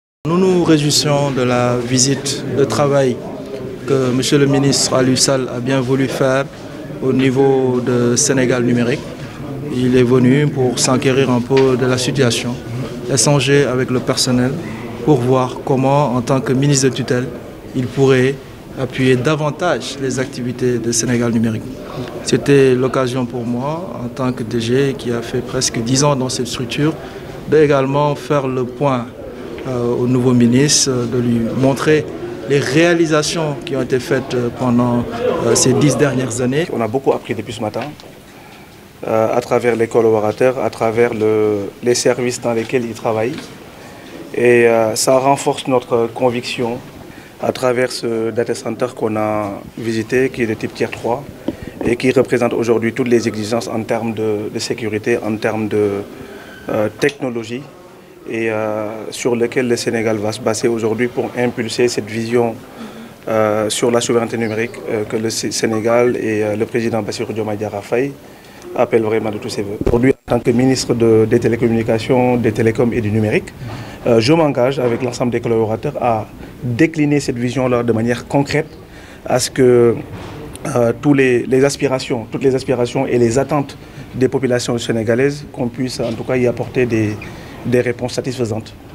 Ministre Alioune Sall visite les centres numériques : Une impulsion vers la souveraineté numérique
Le nouveau ministre de la communication des télécommunications et du numérique, Alioune Sall, a entrepris ce lundi 29 avril une visite des centres numériques de Colobane, Technopole de Pikine et le Data center de Diamniadio.
Cette démarche vise à établir un contact direct avec le personnel et le directeur général, Cheikh Bakhoum, qui se dit prêt, avec son équipe, à accompagner les nouvelles autorités pour atteindre la souveraineté numérique.

Le ministre exprime sa satisfaction quant aux ressources technologiques déjà en place. Cela prouve que  "nous ne partirons pas de zéro pour impulser la vision du président de la République Bassirou Diomaye Faye sur la souveraineté numérique" a déclaré le nouveau ministre de la communication des télécommunications et du numérique.